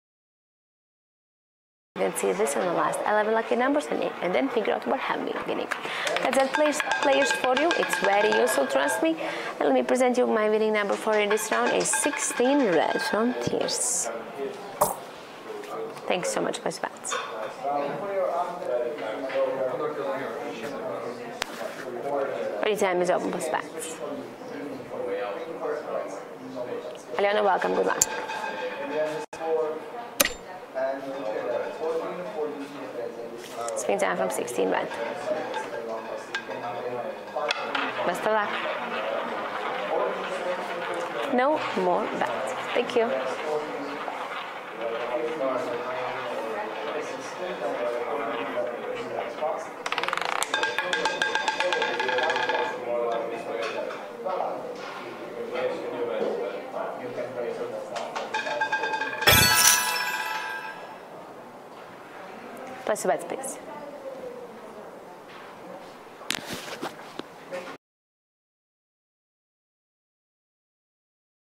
NetEnt Rapid Roulette
Fast spin live dealer roulette tables are all the rage these days. This is NetEnt's fast spin table. Rapid Roulette they call it. This round payed at Unibet.